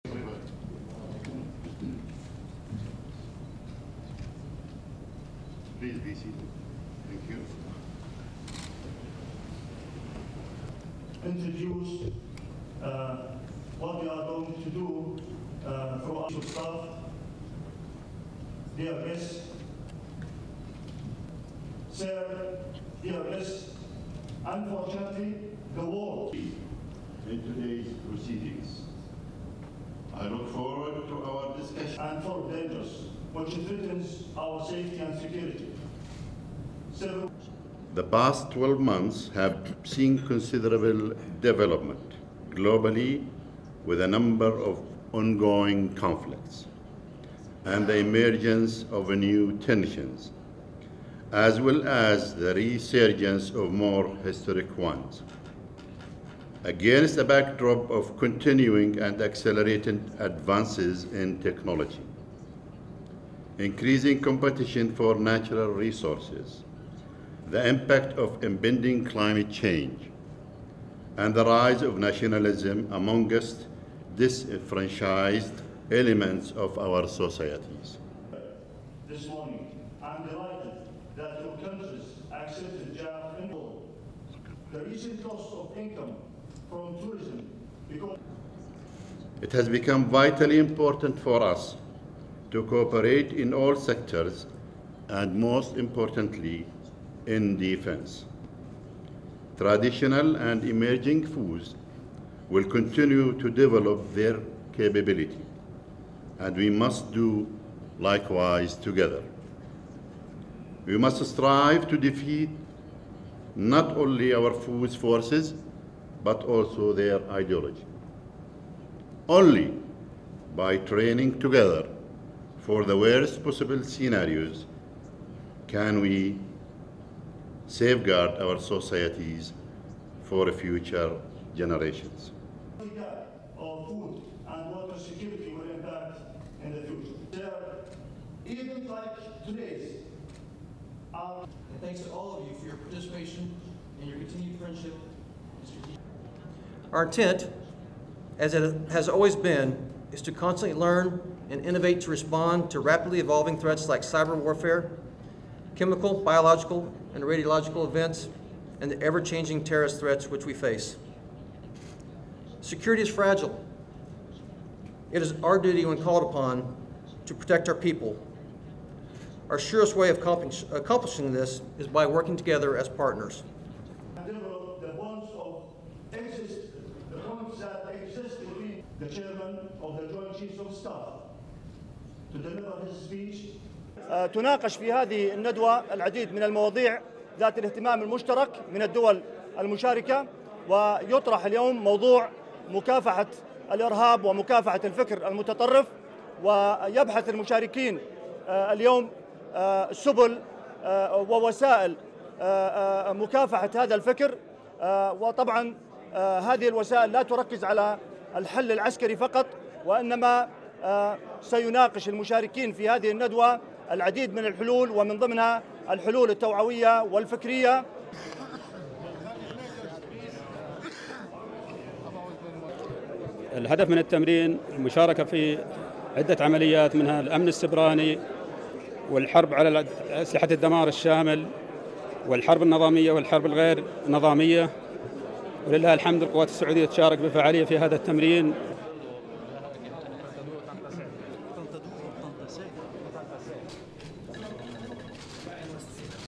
Senior military commanders meet for annual drill
(4 Sep 2019) LEAD IN:
Senior military commanders from around the world are in Jordan for the annual Eager Lion drill.
Their focus is on learning how to adapt to the ever-changing face of modern warfare.

STORY-LINE:
The whole room stands as Major General Yousef Huneiti, Chairman of the Joint Chiefs of Staff in the Jordan Armed Forces, and Major General Bradley A. Swanson, US Central Command's Director of Exercises and Training, enter the room.
This seminar is packed with senior commanders and military experts from 29 countries.
They are in Amman as part of the annual "Eager Lion" drill.
It's a chance for the officers to discuss how their militaries should operate amid ongoing conflicts and global challenges - and how they can work together.
"It has become vitally important for us to cooperate in all sectors and most importantly in defence," says Huneiti.
"Traditional and emerging foes will continue to develop their capability. And we must do likewise together. We must strive to defeat not only our foes' forces, but also their ideology. Only by training together for the worst possible scenarios can we safeguard our societies for future generations."
Major General Bradley A. Swanson, US Central Command's Director of Exercises and Training, says militaries must learn to tackle "rapidly evolving threats" including cyber warfare, chemical, biological and radiological events.
"Security is fragile," he adds.
The Eager Lion 2019 drill started last week with the participation of 8,000 troops.
Participating countries practise how to work together towards military goals, including anti-terrorism operations, as well as procedures for command and control and cybersecurity.
"The goal from the drill is to participate in various operations such as cybersecurity, war against weapons of mass destruction, the conventional and non-conventional war, and thanks to Allah, the Saudi forces are actively involved in this exercise," says Major General Ahmed al-Moqrin, deputy commander of Royal Saudi Land Forces.
The Eager Lion has been held in Jordan since 2011.
The drill will conclude on September 5.